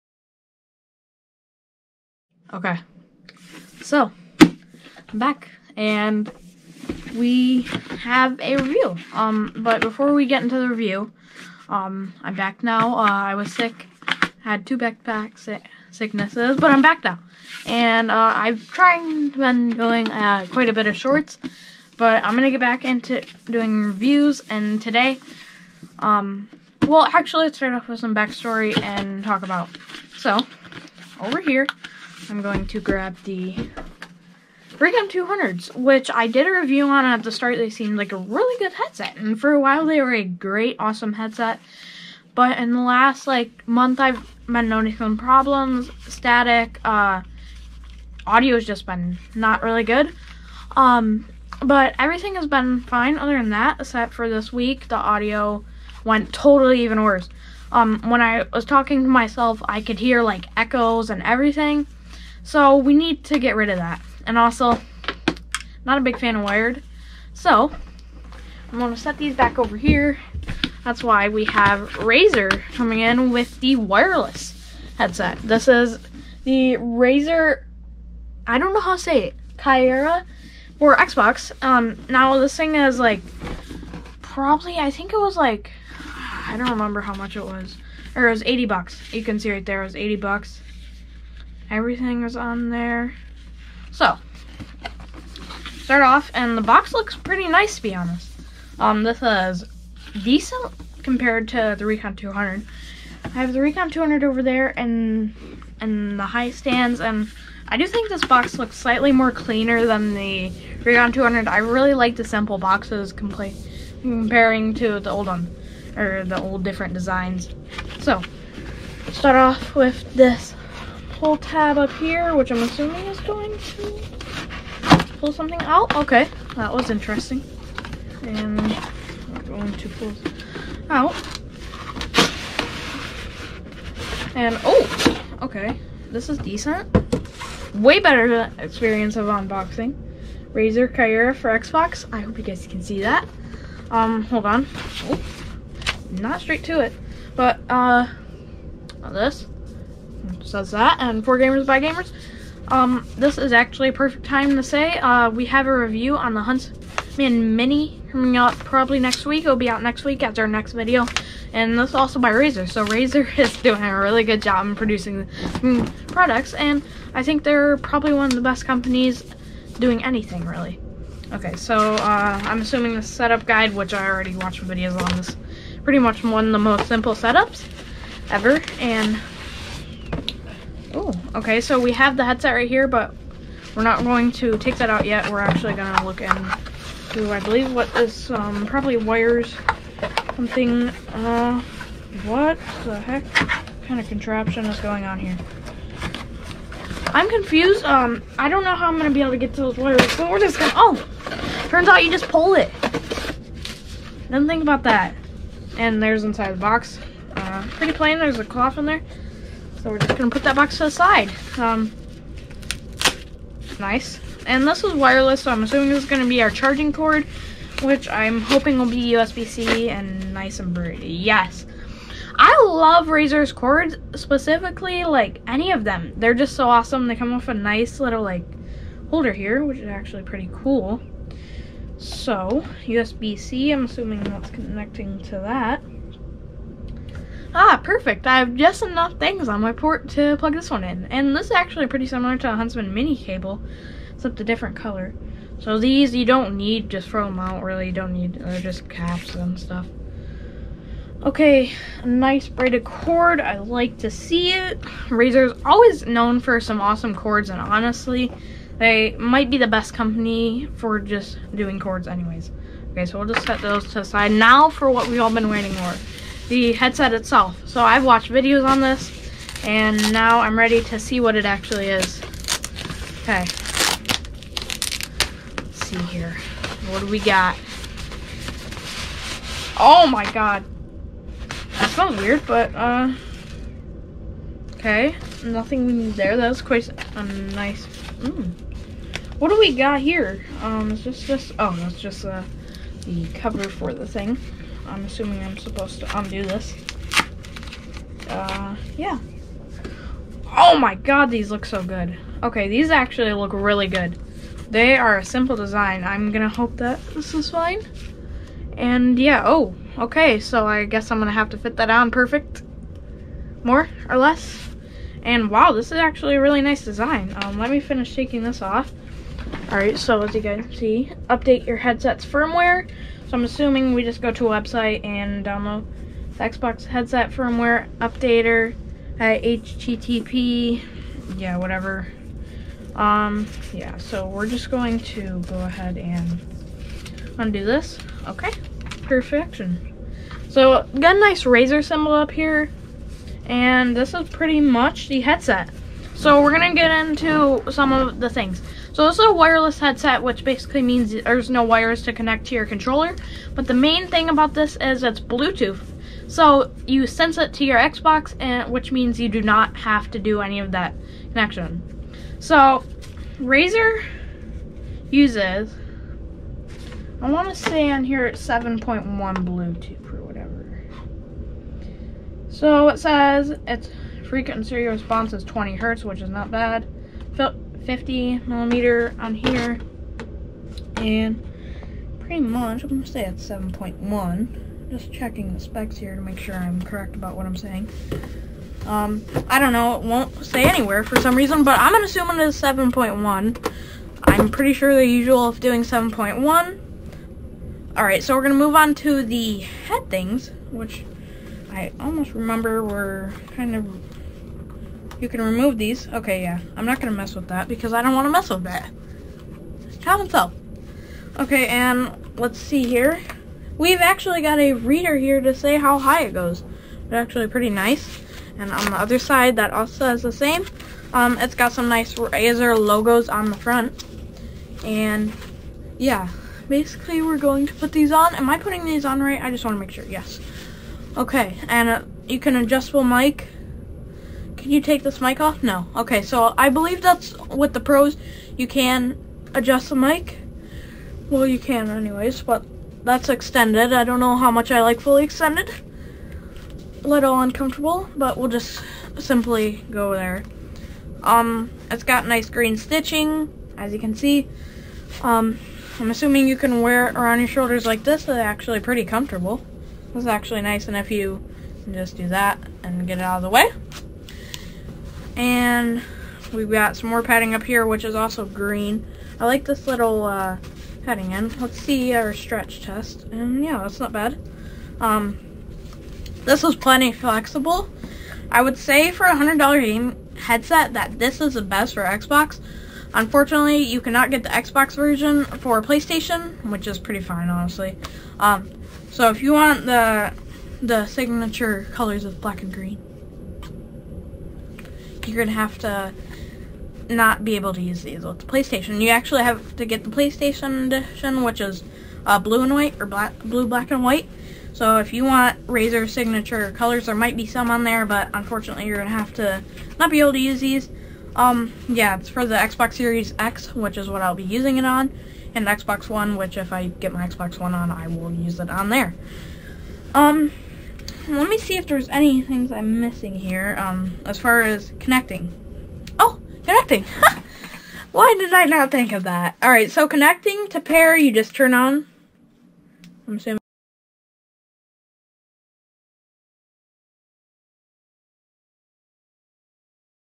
Razer Kaira (Unboxing and Review)
The razer kaira is a gaming headset for Xbox.
It’s is wireless using Bluetooth.